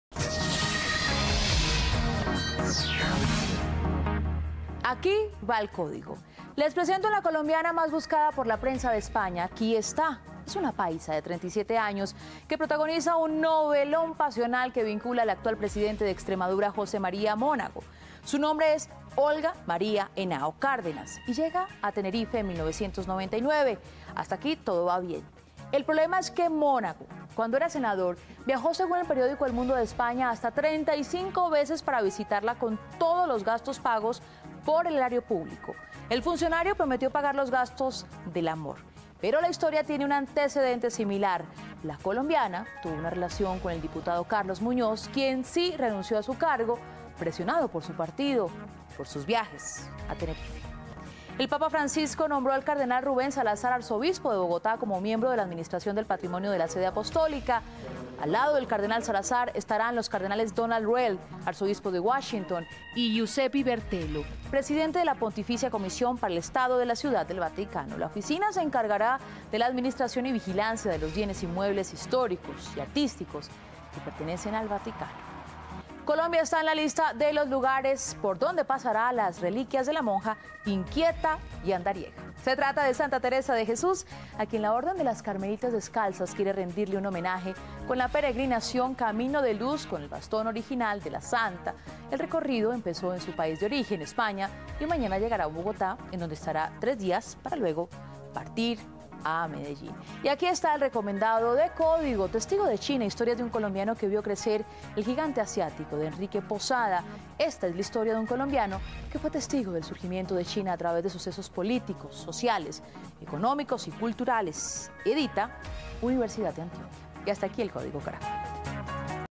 Un diputado español está en líos por su amor a una paisa
Emitido el 11 de Noviembre de 2014. Con cargo al erario, José Monago hizo más de 30 viajes a Canarias por ver a la colombiana Olga María Henao. Ahora tendrá que devolver el dinero.